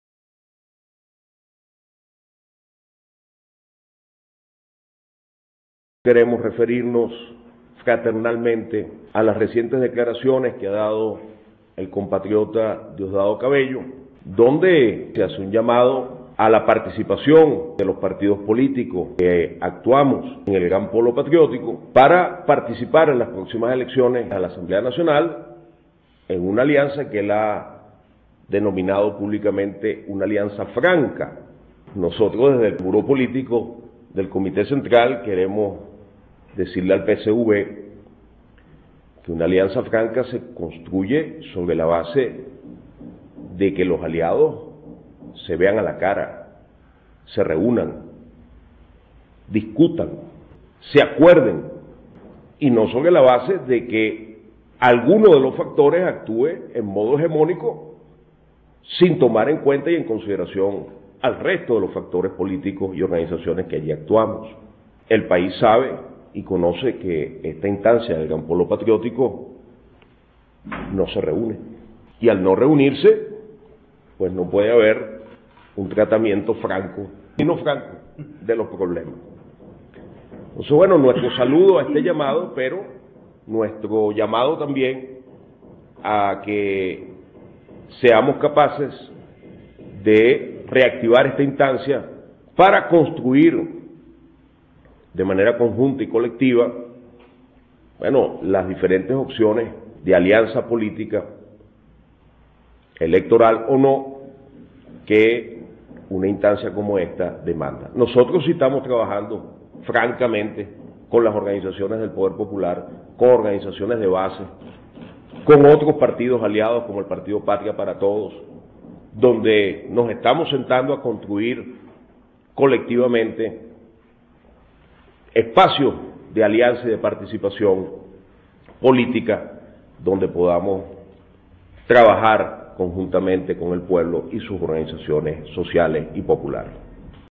Exigimos alianza para construir la salida revolucionaria a la crisis
02-Mar-2020
Vocero: Carlos Ojeda Falcón
Miembro del Buró Político del Partido Comunista de Venezuela (PCV)